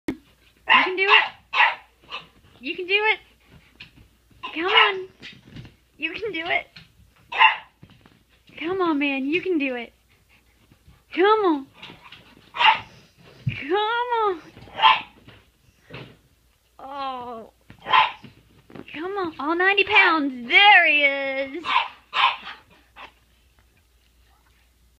Qtip's Dog Enzo Climbing into bed.....
My Dog Enzo doesn't know how to Jump!!!. This is how he gets on the bed every night. He's big enough to walk up onto the couch, but he seriously can't jump onto/into anything. I have to lift him into my truck...Onto the bed and all!